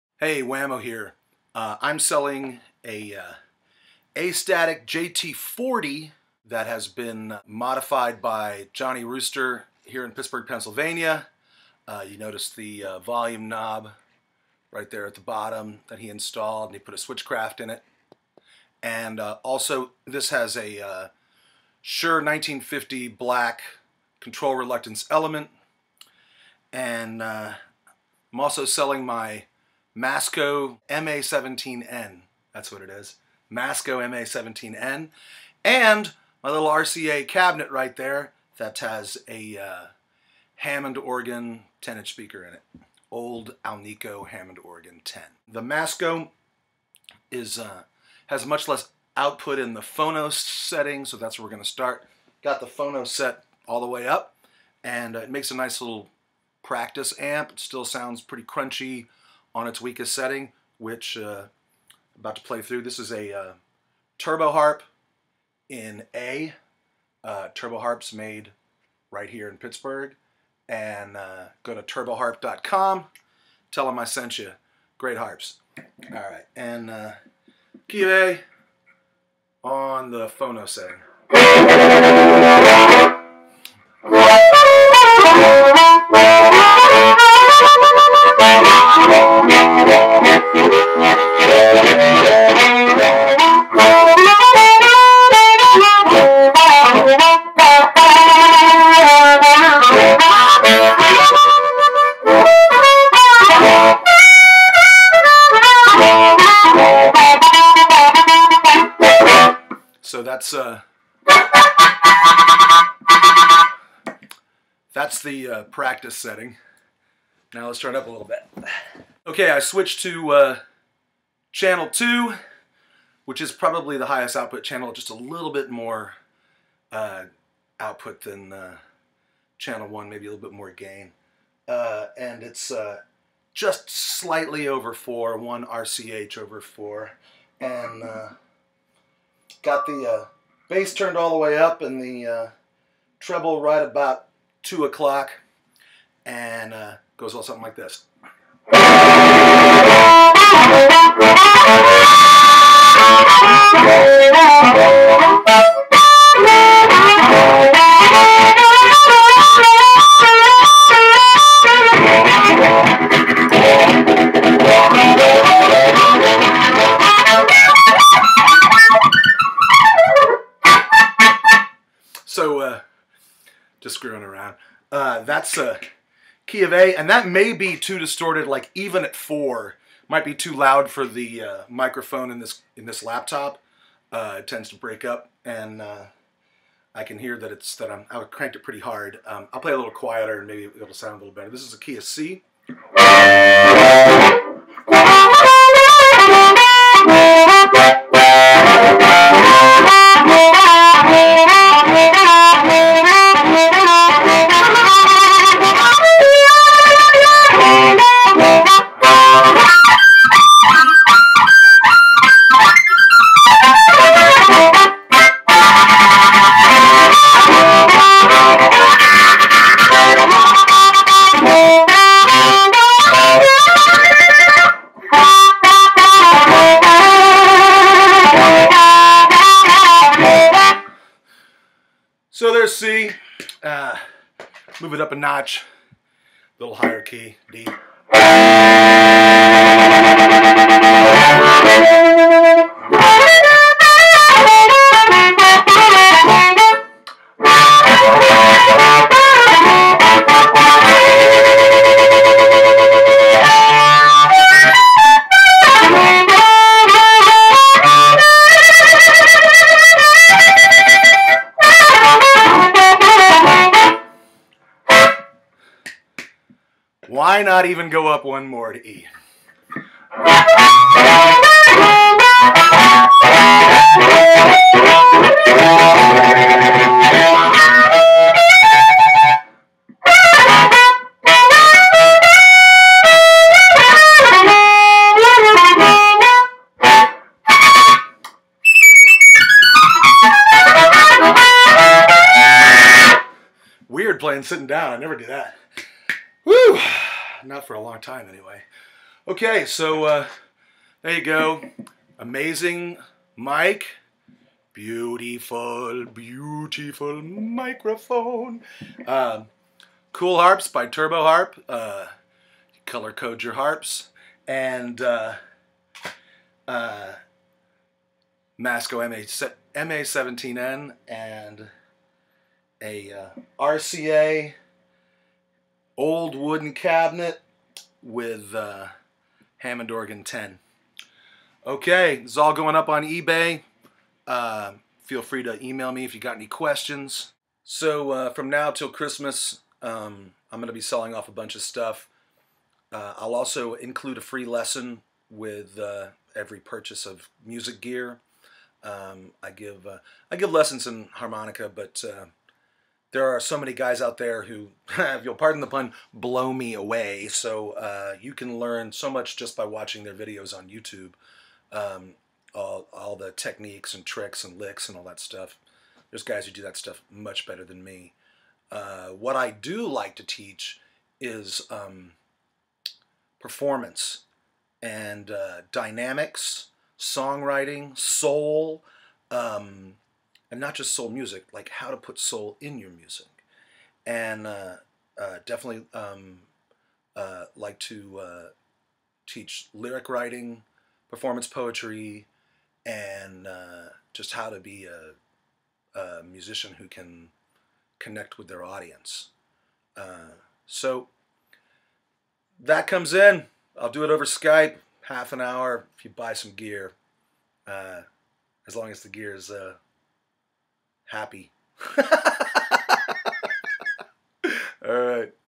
Astatic CR JT-40, Masco MA-17N, RCA Cabinet, Wammo!
I'm selling my Astatic JT-40 mic with a Shure black label, controlled reluctance microphone element dated April, 1950 AND a Masco 17N PA Amplifier AND an RCA speaker cabinet with a Hammond Organ alnico 10" speaker. This is all amazing, vintage stuff and all up on ebay as of 11/19/12.
Here's the microphone
Here's the Masco Amp
Here's the RCA cabinet
Enjoy! Thanks for looking.